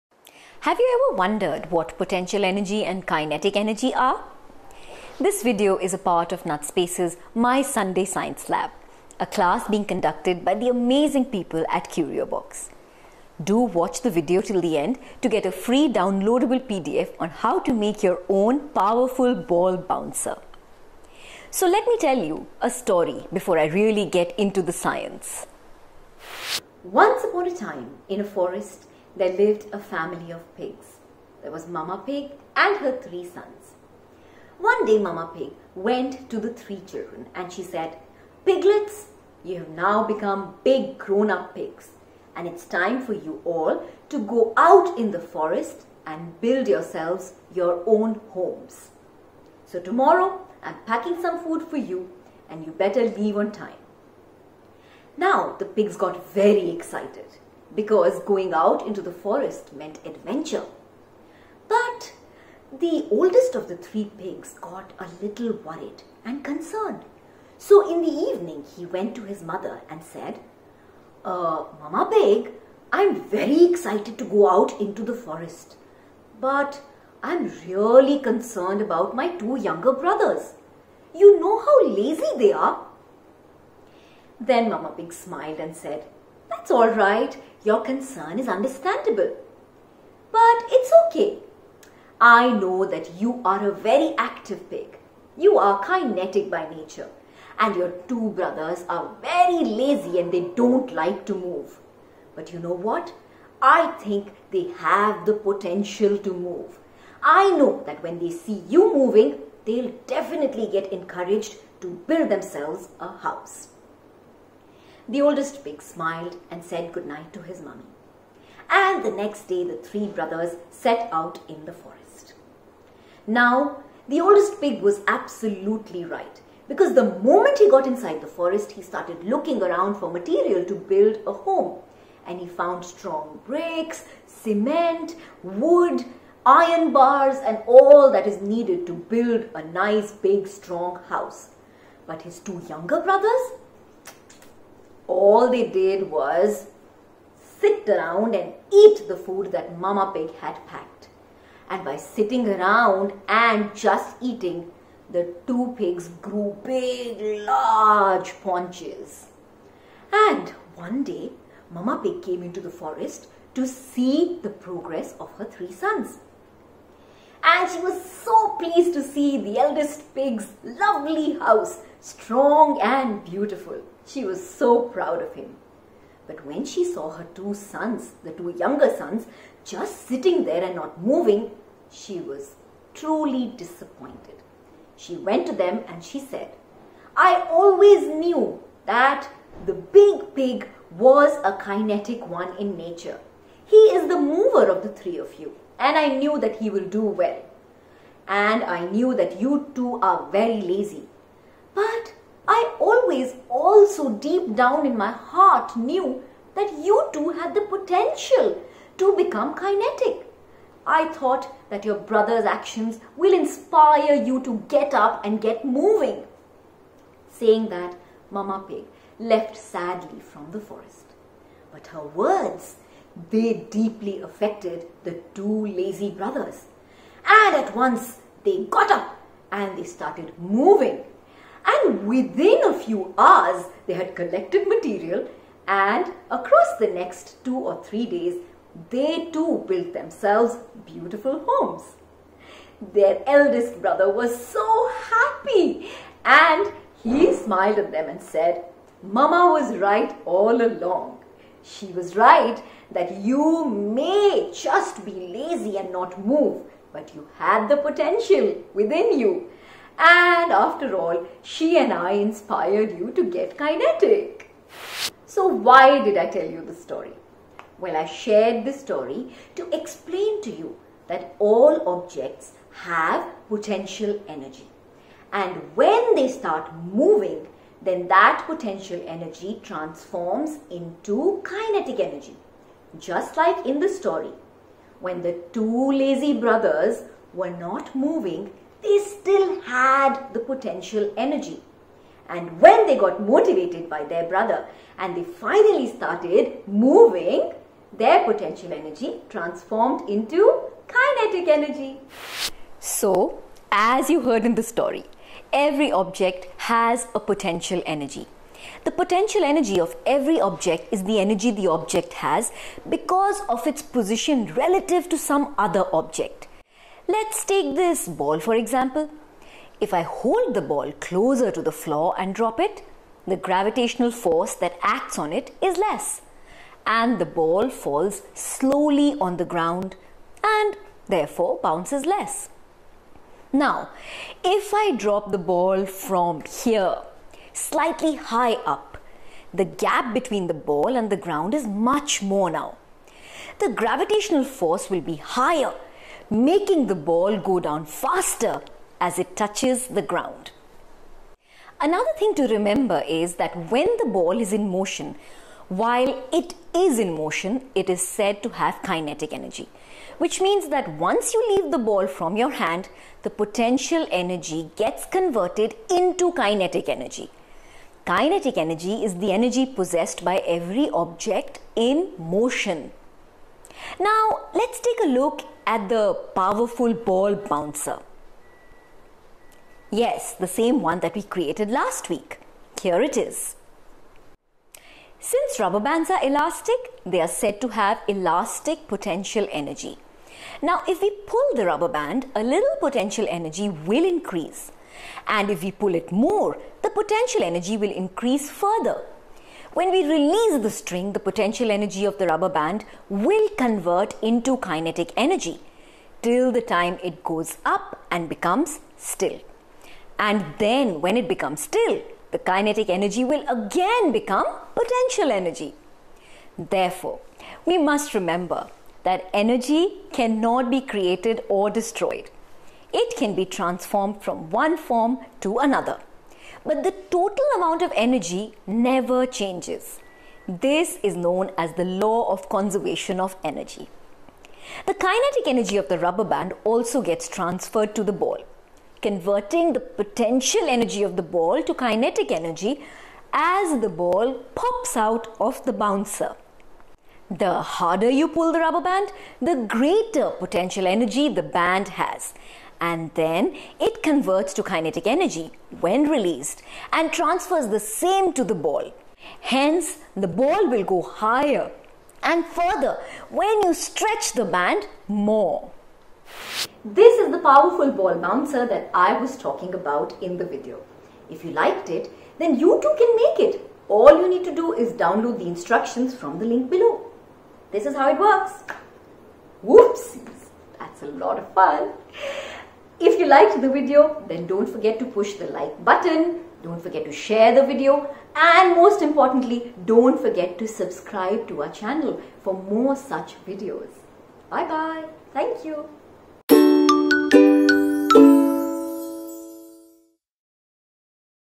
Kinetic & Potential Energy explained with the Story of Three Little Pigs
This video makes children understand the concept of Potential Energy & Kinetic Energy with help of a story and a few examples and experiments.

Learn how to make the Powerful Ball Bouncer by downloading the instructions

What is Potential Energy?
Every object has potential energy. The potential energy of every object is the energy the object has because of its position relative to some other object.

What is Kinetic Energy?
Kinetic energy is the energy possessed by every object in motion.

What is Law of Conservation of Energy?
The law of conservation of energy is a law of science that states that energy cannot be created or destroyed, but only changed from one form into another or transferred from one object to another.

Understand all these concepts by listening to a story of 3 Little Pigs who are sent by their mother to build homes for themselves. One of the pigs is kinetic and quickly builds his house, while the other two are lazy, but their mother has faith in them and she knows that the other two pigs also have the potential to build beautiful homes for themselves.

Key Moments:
0:00:25 Story to Understand Kinetic & Potential Energy
0:05:02 What is Potential Energy?
0:05:52 What is Kinetic Energy?
0:06:20 Ball Bouncer Experiment
0:06:57 Law of Conservation of Energy
0:07:48 Ball Bouncer Demo